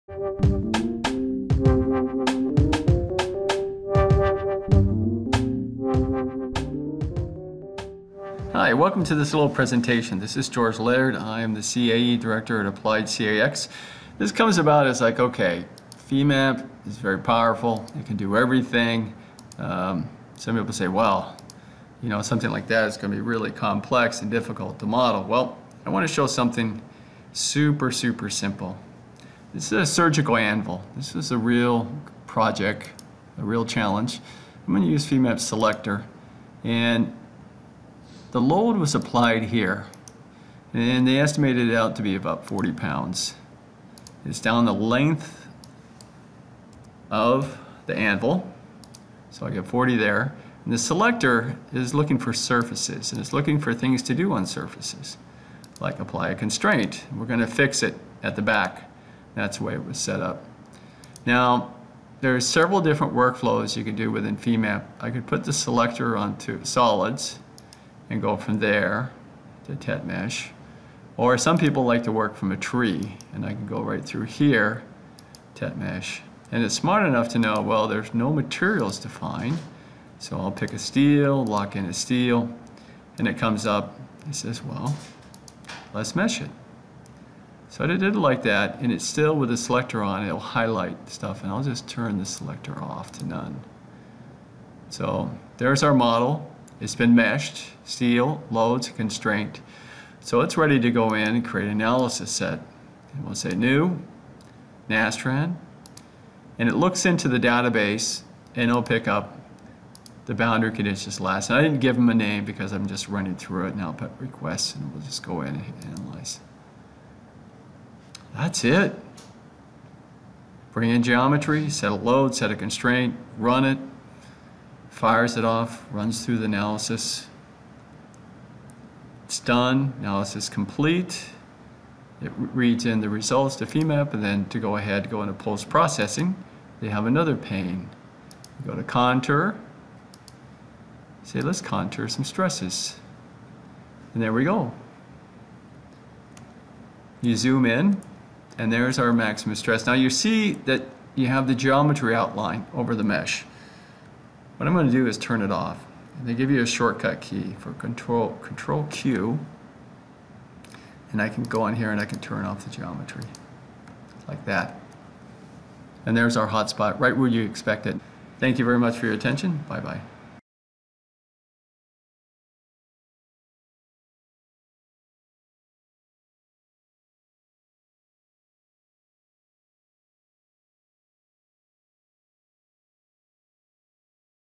CAD to FEA Super Simple in Three Minutes
Hi, welcome to this presentation. We created this to show that yes, FEMAP is very powerful, but it doesn’t mean it's going to be really complex and difficult to model.

In this video we show something super, super simple. It's a surgical anvil. This is a real project. We cover use of the FEMAP selector, applying a constraint, and the several different workflows you can do within FEMAP. We pick our material, adjust our selector, and quickly get to our mesh & run our analysis.

And that's it. Bring in geometry, set a load, set a constraint, run it, fires it off, runs through the analysis, and it's done, analysis complete. It reads in the results to FEMAP, then we go into post processing, we contour some stresses and there we go. Lastly we turn off the geometry, and we find our hotspot, right where you'd expect it.